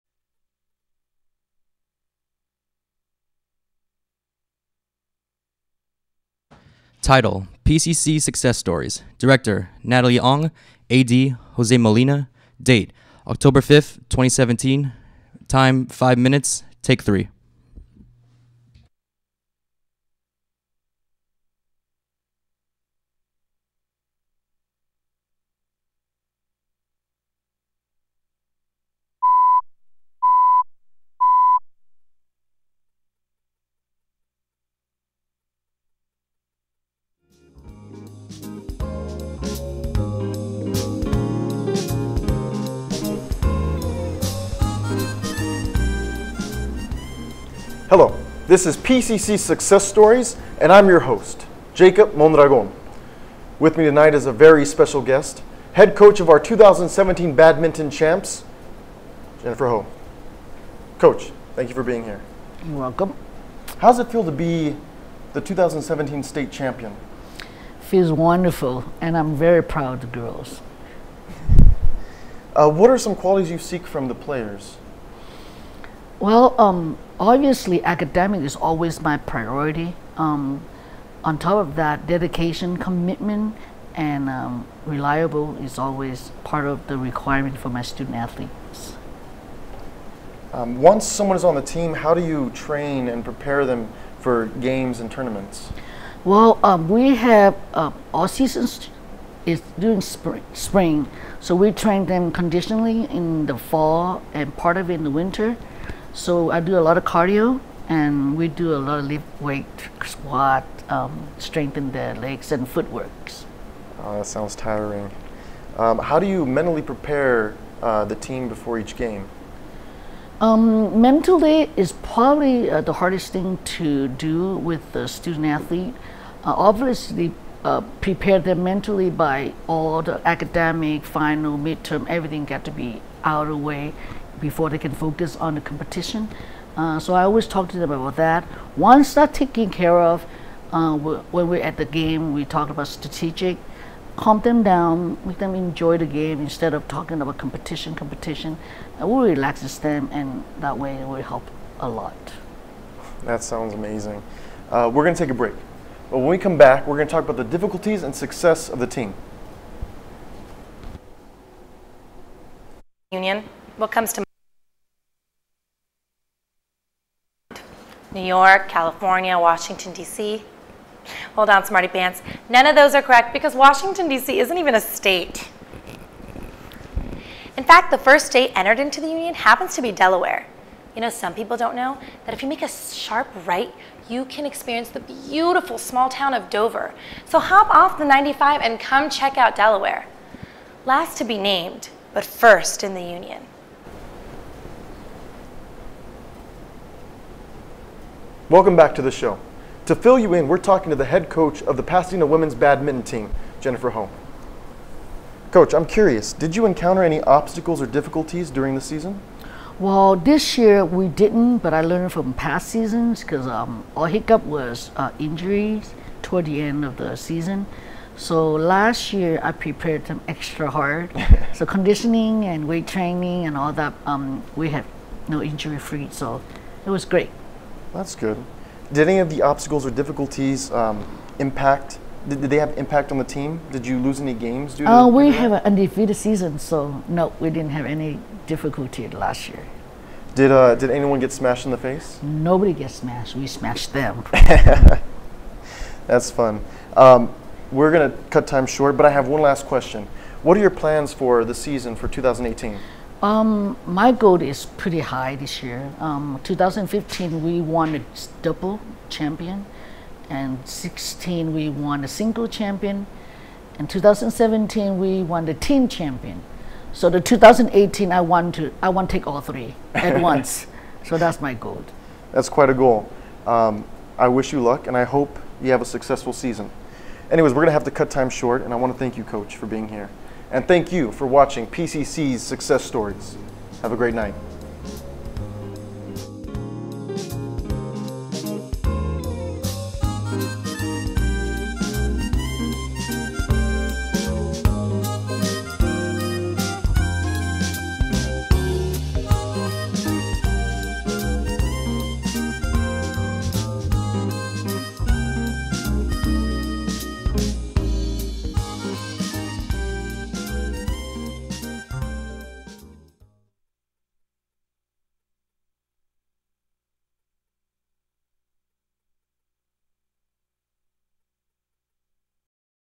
PCC success stories
Interview with one of the award winning PCC badminton coaches. Attractive lighting design, effective depth staging.